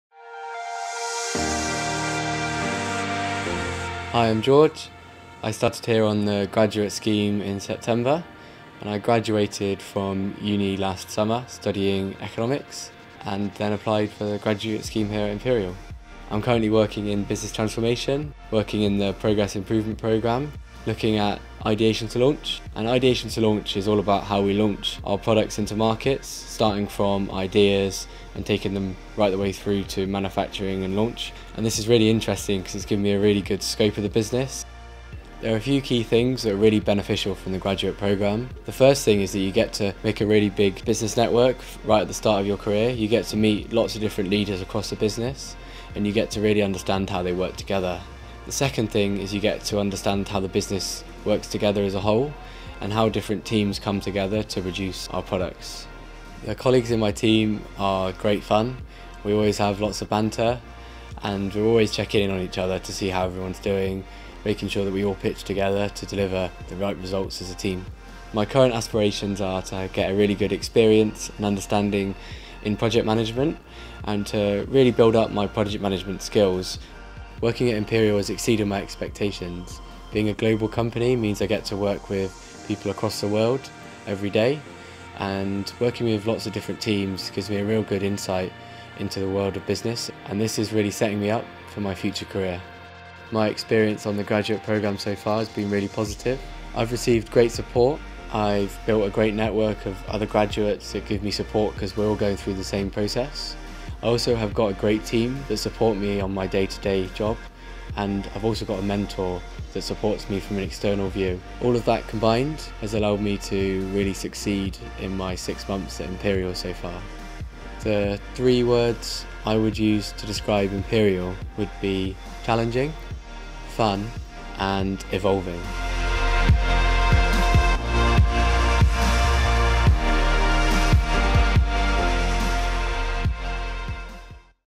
Meet George - Graduate: Business Transformation
Find our more about our Graduate Programmes. Here, George talks about his  role in Business Transformation as part of our Graduate Rotational Programme.